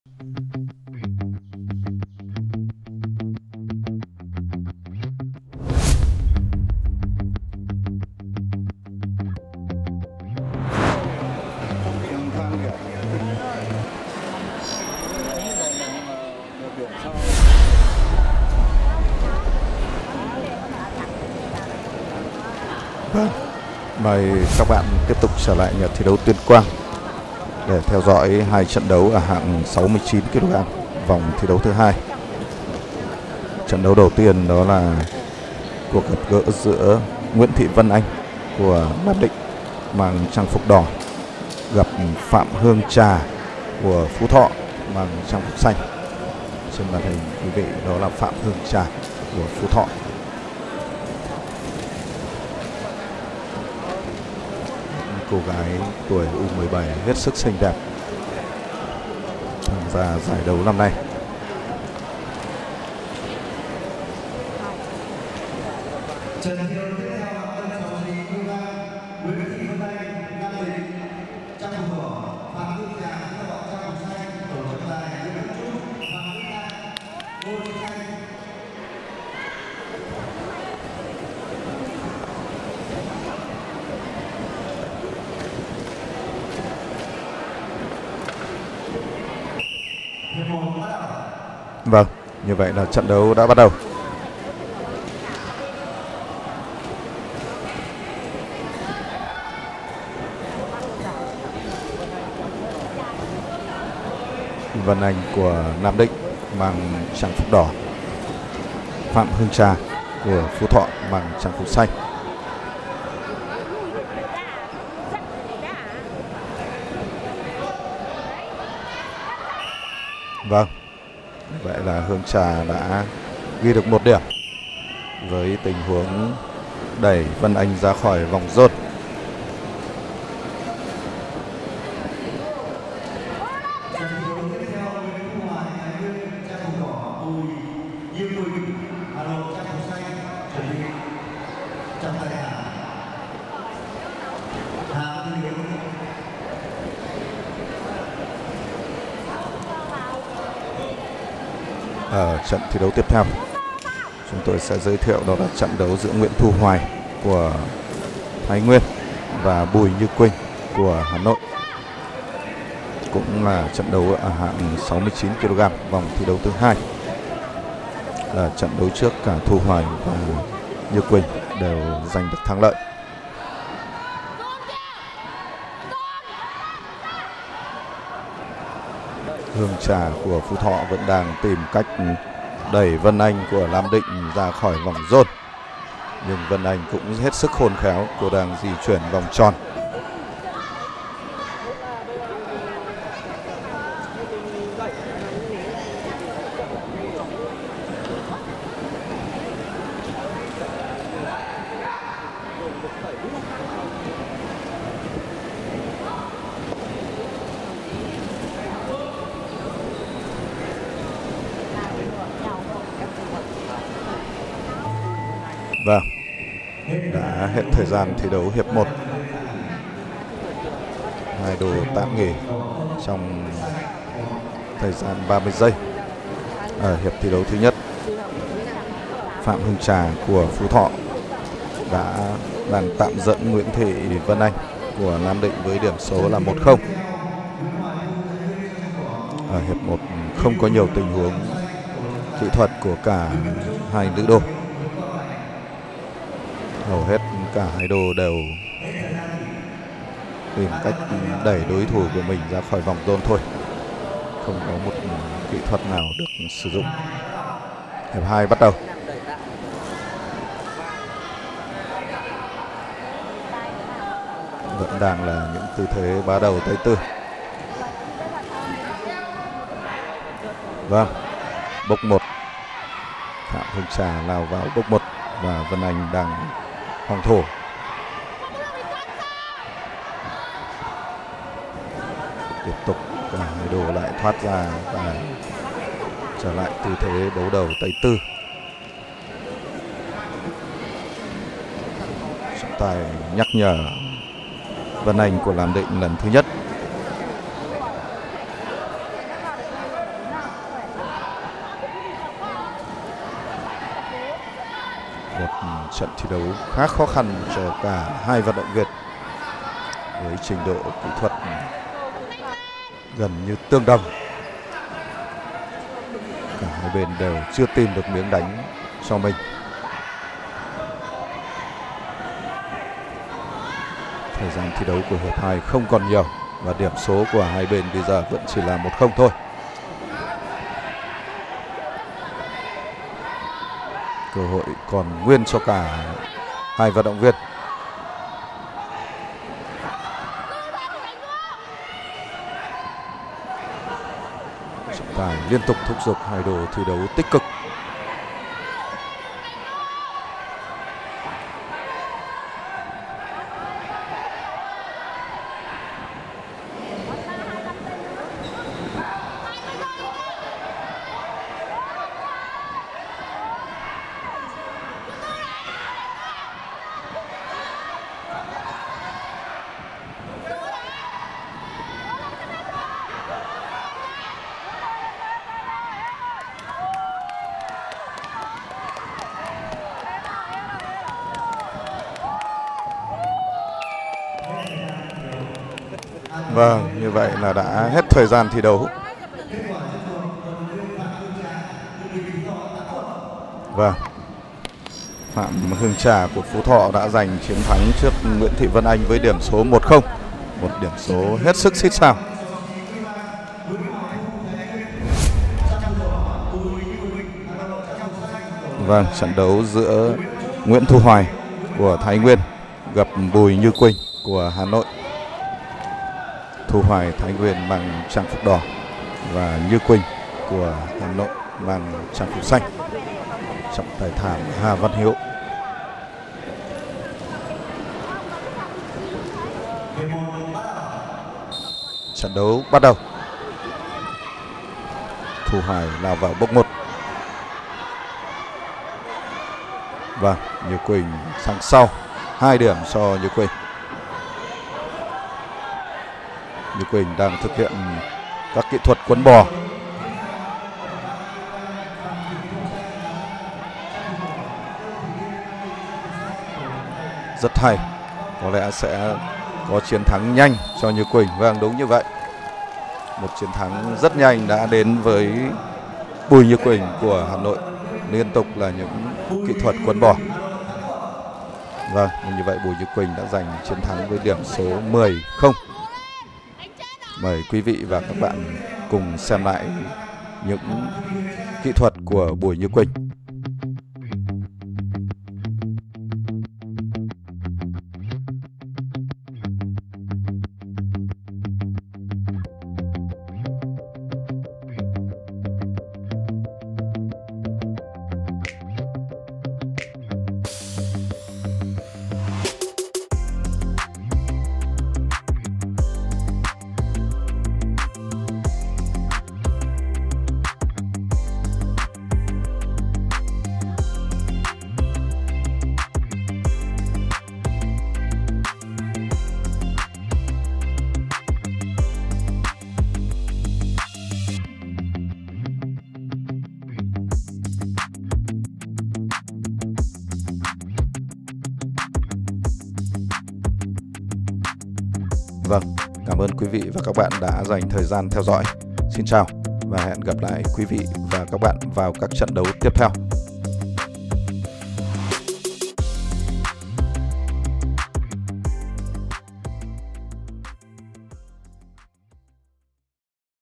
❤️ Vật nữ trẻ Tuyên Quang - Trận 61, 62 - hạng 69 kg. Vòng 2 . women wrestling.
vatdantoc vatnu vatnutre. Các trận thi đấu giải vô địch quốc gia vật nữ trẻ lứa tuổi u 16 và 17.. được tổ chức tại nhà thi đấu Tuyên Quang. WWE SmackDown